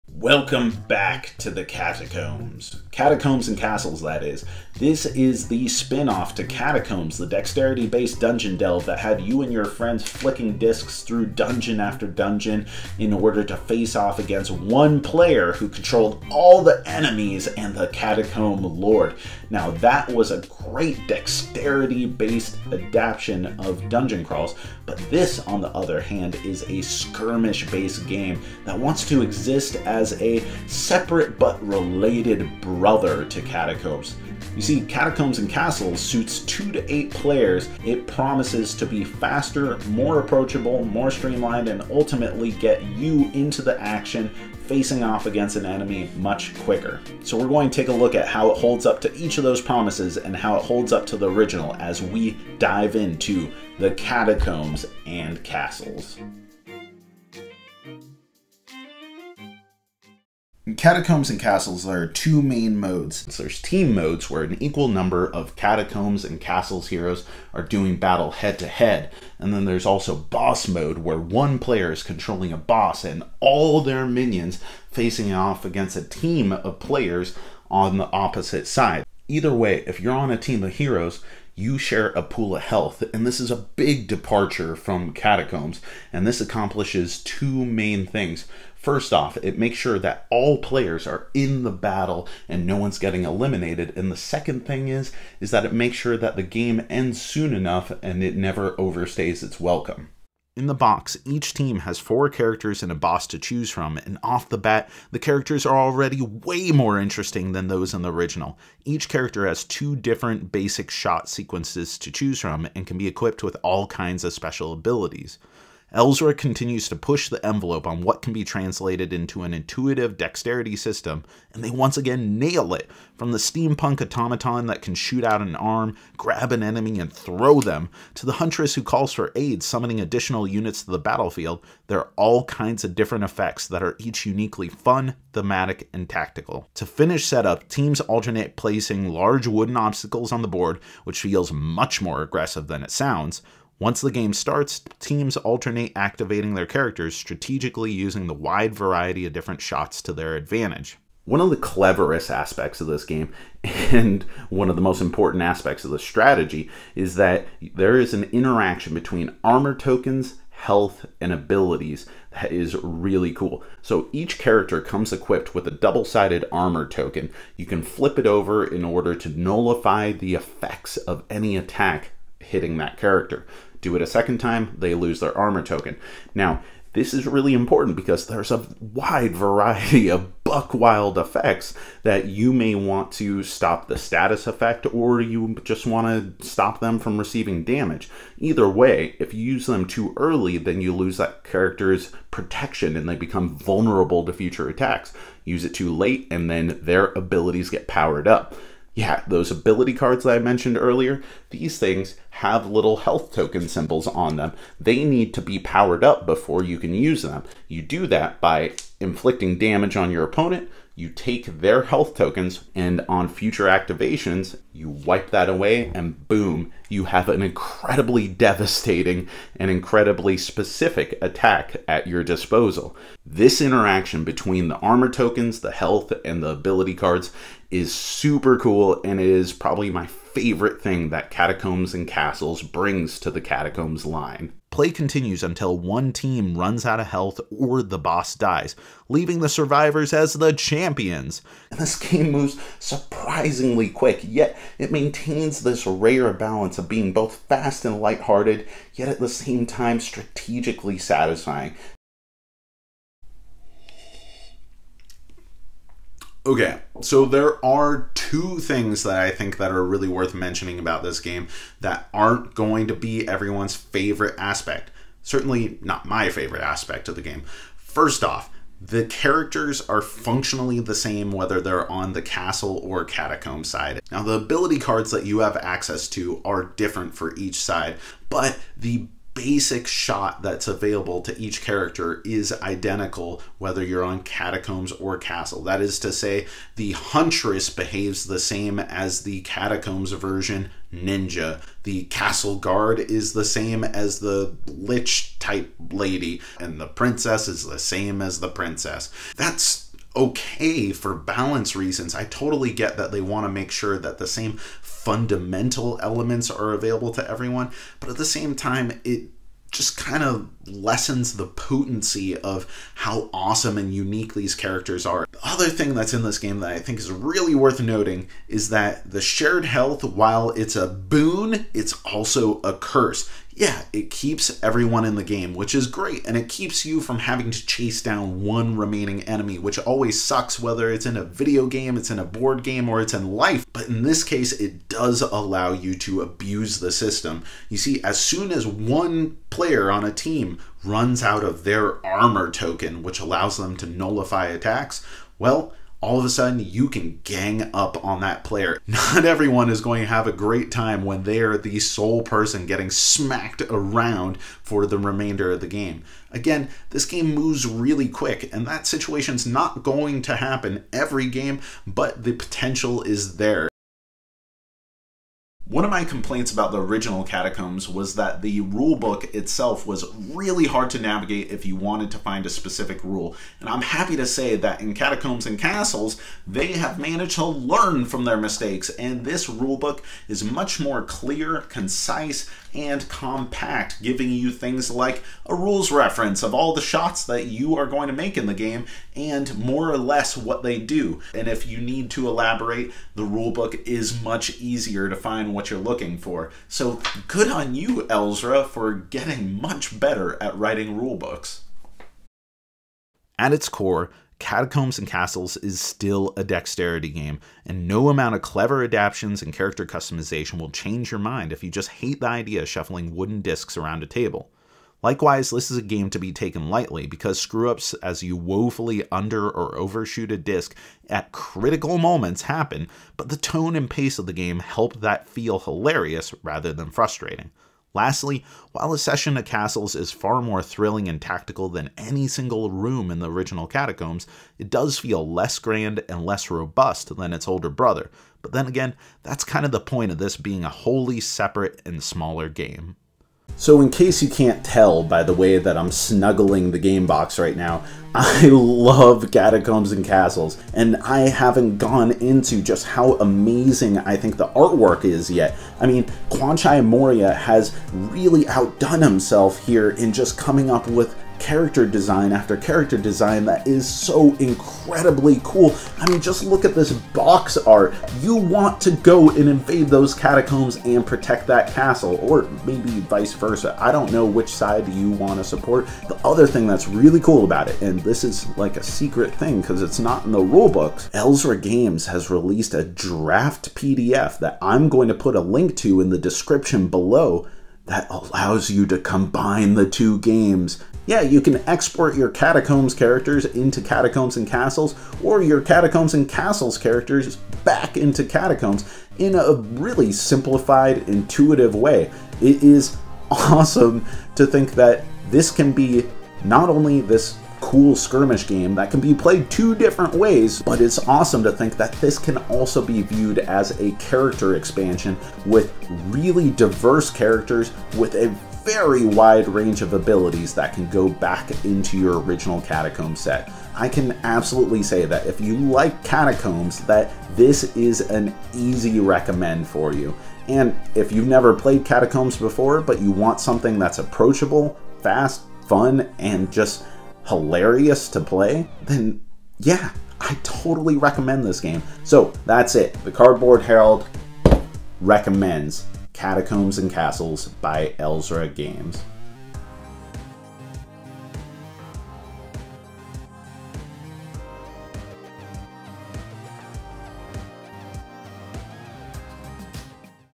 Catacombs & Castles Review
Jack dives back into Catacombs with Catacombs & Castles by Elzra Games. This is the single battle skirmish spinoff to Catacombs, but does it manage to deliver on being a faster, more approachable, and more tactical game?